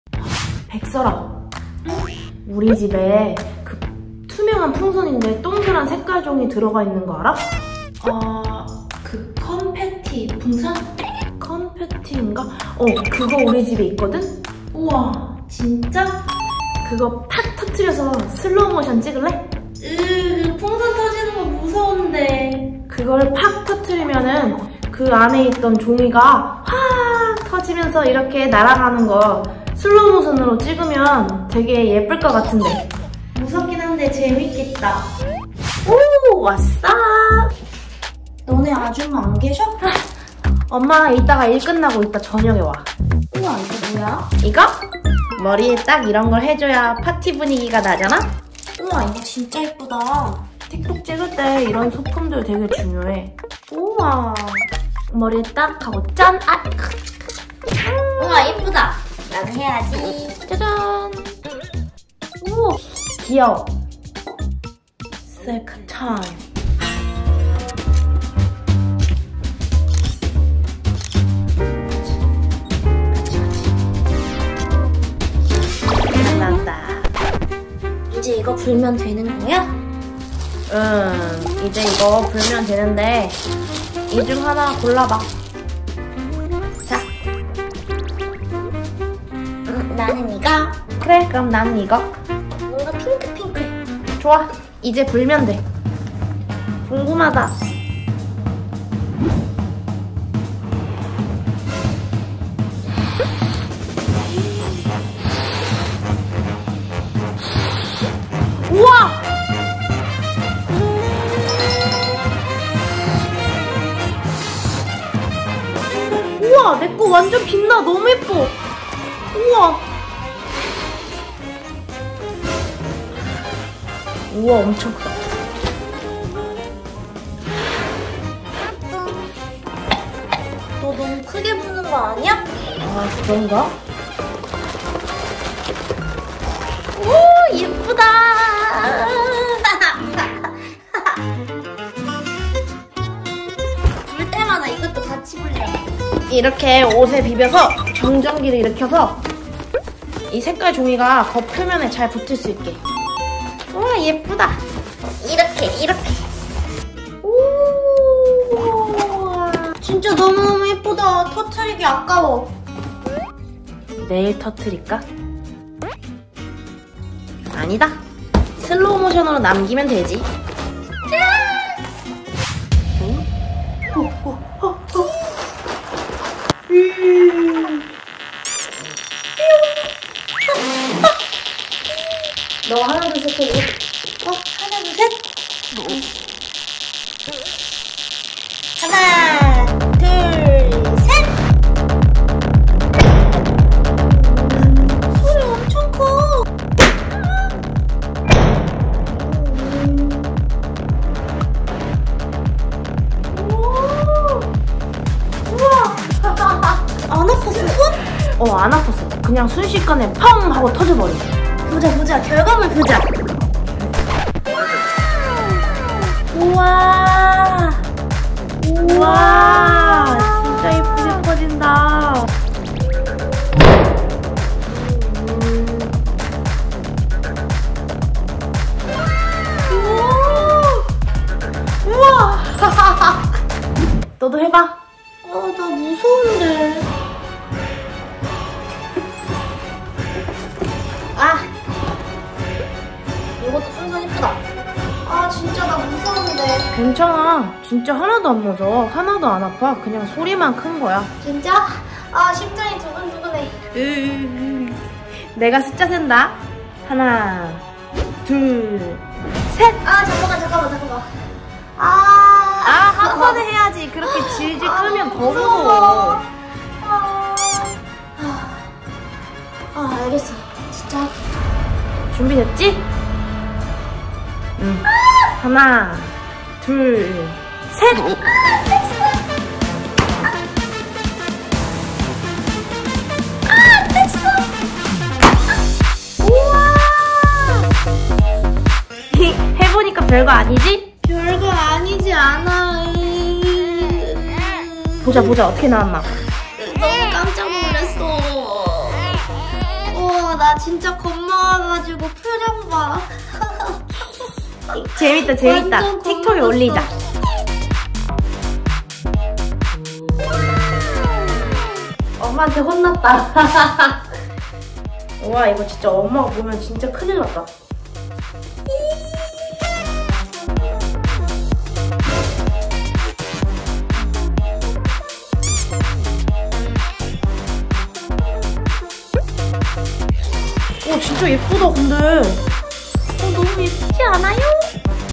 컨페티풍선 터뜨리기 -슬로우모션 찍기!
설아랑 집에서 풍선 터뜨리기 했어요♡
터뜨리기 직전에 엄청 무서워요!
그래두 슬로우모션 찍은 결과물이
너무 멋찌게 나와서 좋아요
여러분도 나중에 한번 해보세요
강추!!!
대신 엄마오기전에 치워야 되요 안그럼 엄청 혼날거에요 ㅜ